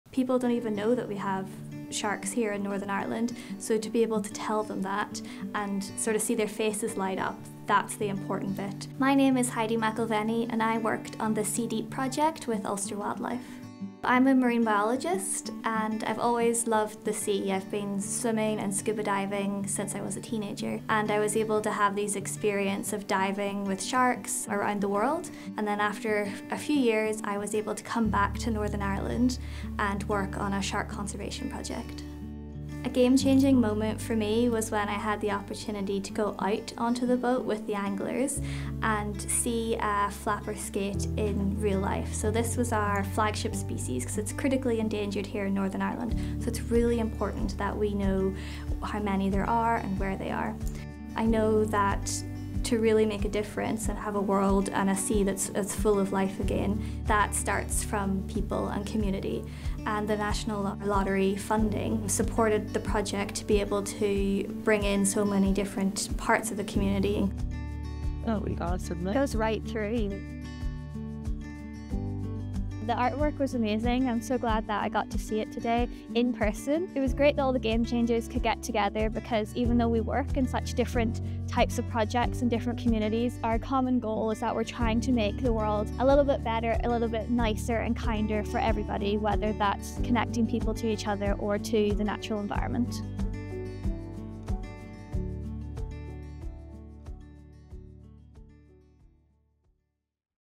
Heidi Mcllvenny - Heritage Game Changer - National Lottery 30
Heidi McIlvenny joined The National Lottery funded ‘Sea Deep’ shark conservation project led by Ulster Wildlife which ran until June 2022. From tagging sharks at sea, to hunting for their egg cases on the shore, they trained and equipped volunteers to help identify important spawning and nursery grounds, to target conservation efforts. The Sea Deep Project launched a first-of-its-kind shark-tagging programme in Northern Ireland to gather vital data about local sharks, skates and rays.

Heidi is a firm believer that the natural environment is a key part of our heritage, and that experience is one of the key tenets of getting people to start to notice and care about the natural environment. She believes conservation projects such as this one help to make that a reality.